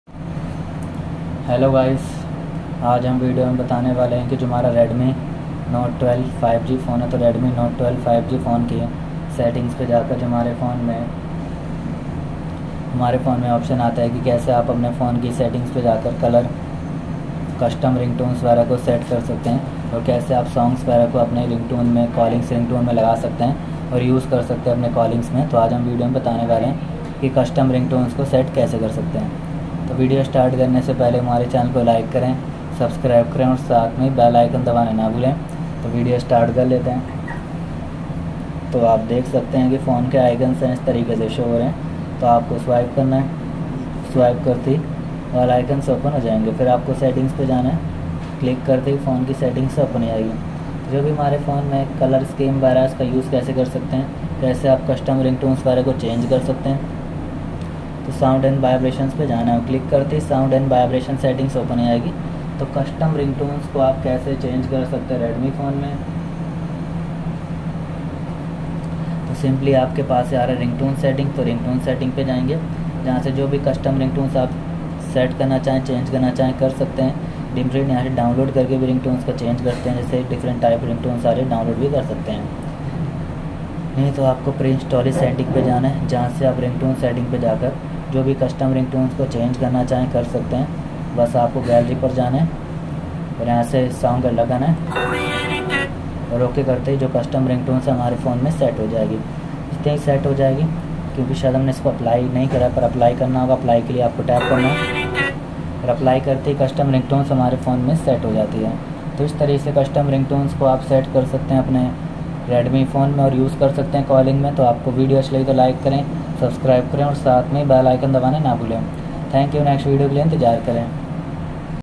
How to set custom ringtone, redmi Note 12 5G me song ringtone Kaise lagaya
Best powerful CPU under 10k

60-Inch Tripod with Bag

7 Socket 6A Extension Board